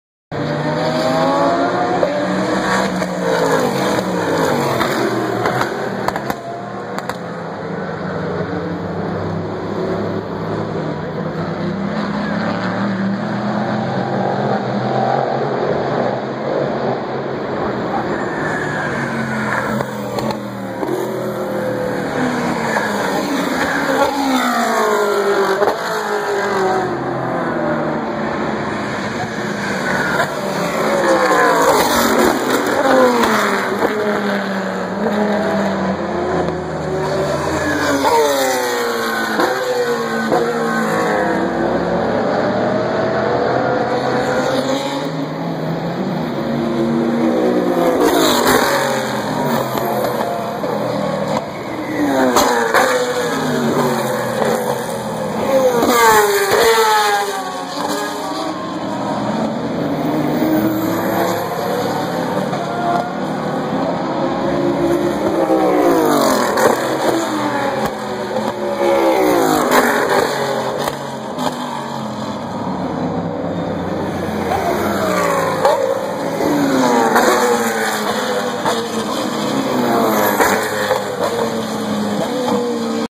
Petit LeMans 2015 Turn 10 Corvette C7r
Petit LeMans 2015 at Road Atlanta full speed off of the straight heading into turn 10